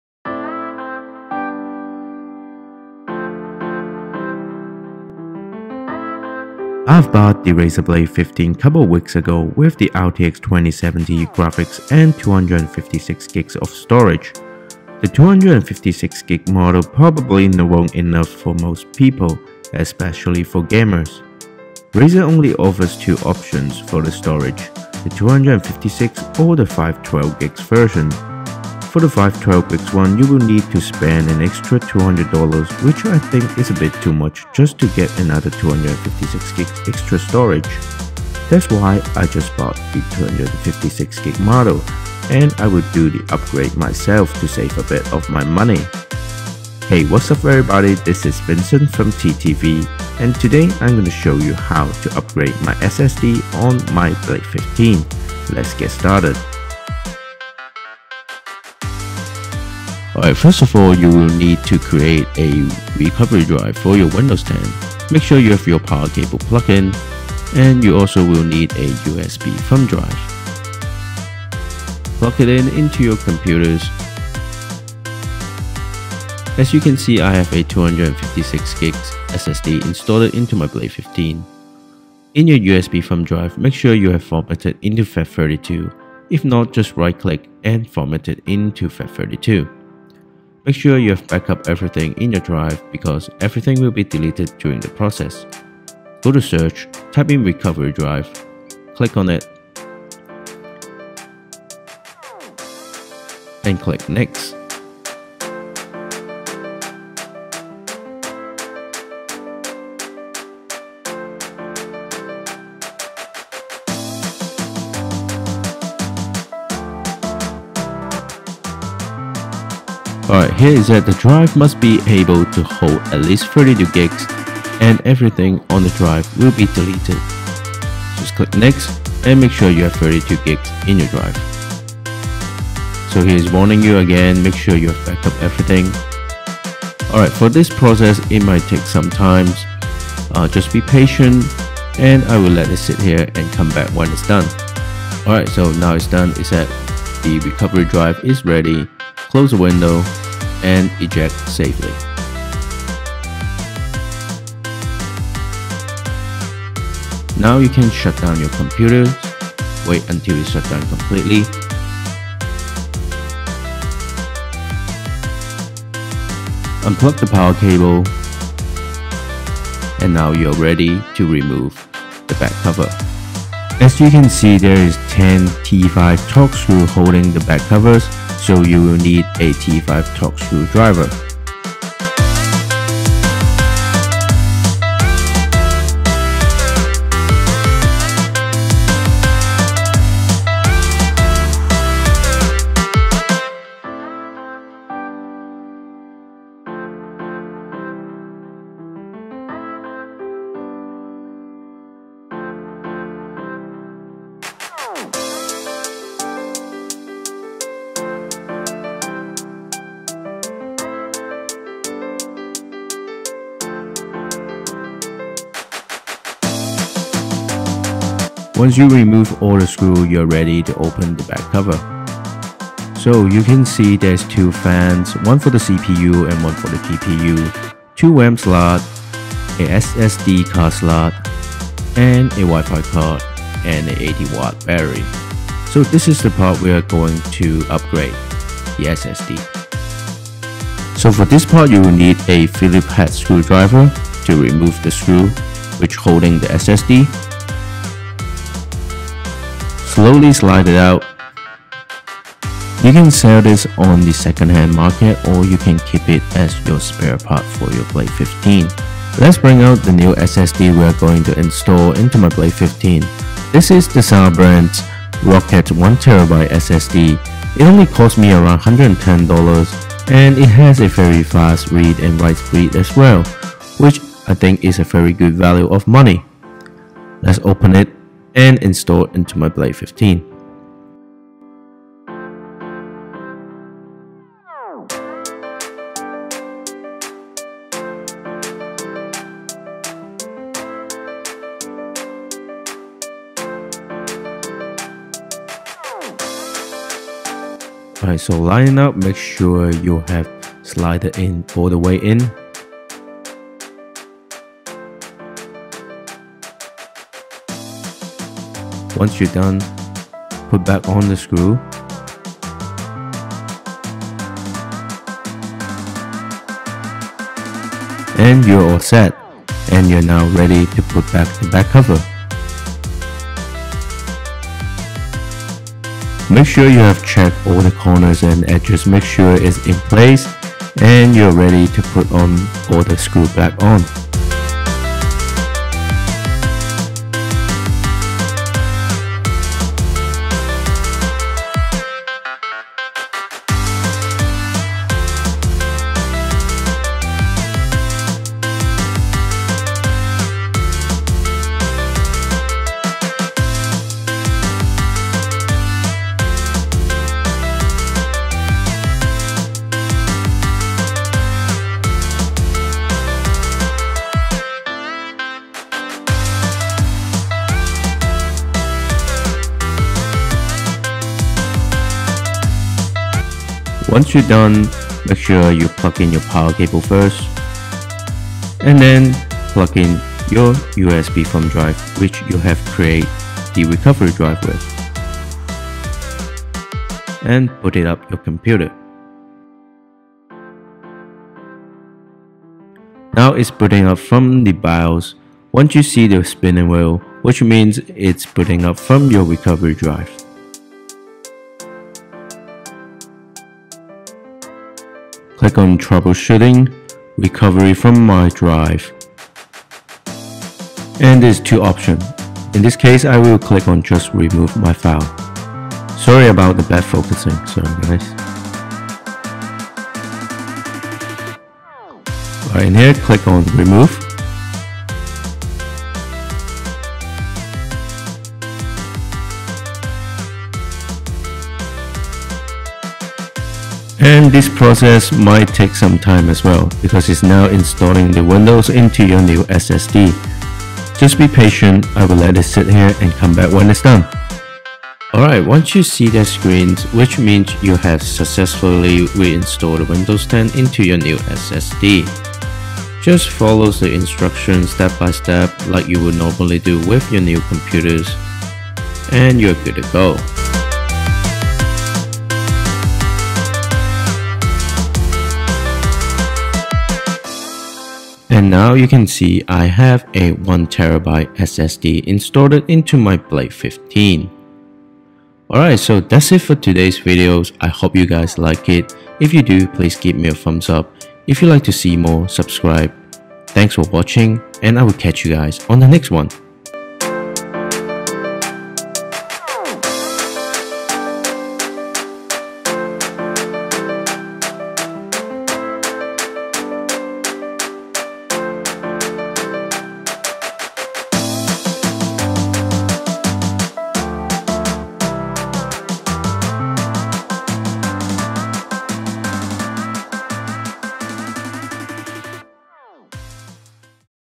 Sabrent Rocket 1TB SSD Upgrade On My 2019 Razer Blade 15 RTX2070 By Creating A Recovery Drive
The Razer blade 15 has been a great device to me, but Razer only offers 256 or 512Gb of storage, so I think it will be a good idea to buy the cheaper 256gb version and upgrade it myself to save some money.

Sabrent Rocket 1TB Nvme SSD:

Razer Blade 15: